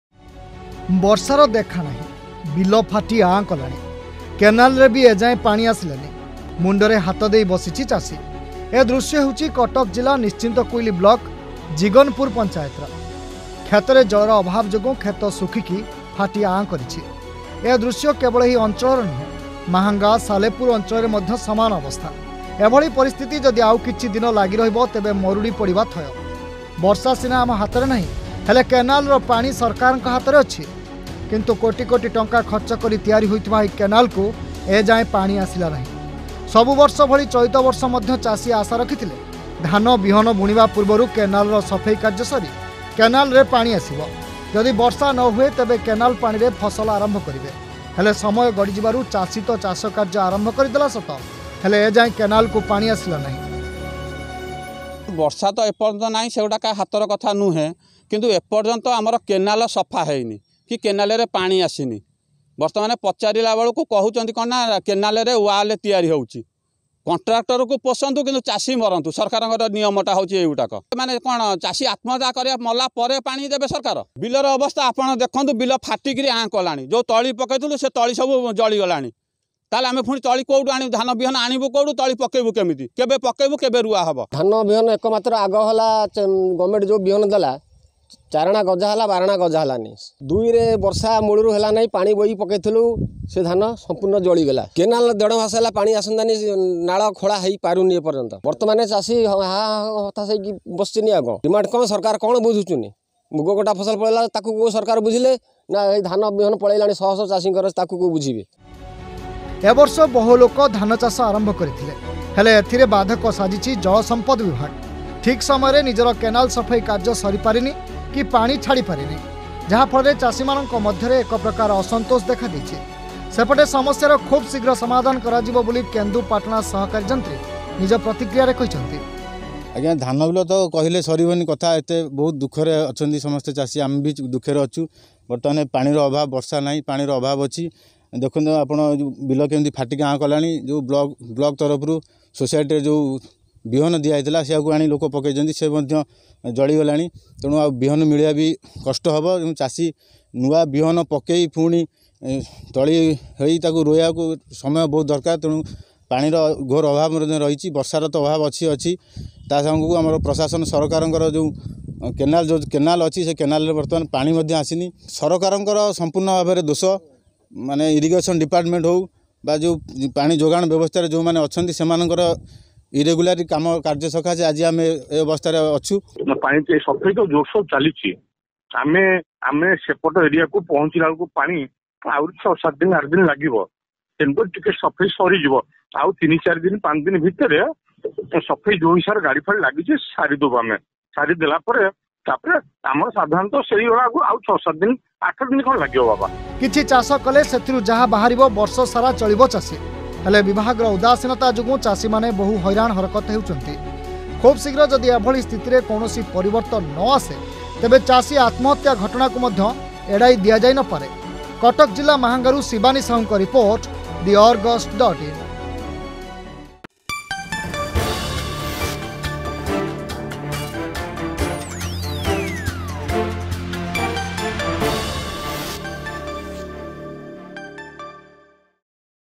ବର୍ଷାର ଦେଖା ନାହିଁ... ବିଲ ଫାଟି ଆଁ କଲାଣି.....|theArgus
ବର୍ଷାର ଦେଖା ନାହିଁ... ବିଲ ଫାଟି ଆଁ କଲାଣି । କେନାଲରେ ବି ଏଯାଏଁ ପାଣି ଆସିଲା ନାହିଁ । ମୁଣ୍ଡରେ ହାତ ଦେଇ ବସିଛି ଚାଷୀ । ଏ ଦୃଶ୍ୟ ହେଉଛି କଟକ ଜିଲ୍ଲା ନିଶ୍ଚିନ୍ତକୋଇଲି ବ୍ଲକ ଜିଗିନପୁର ପଞ୍ଚାୟତର.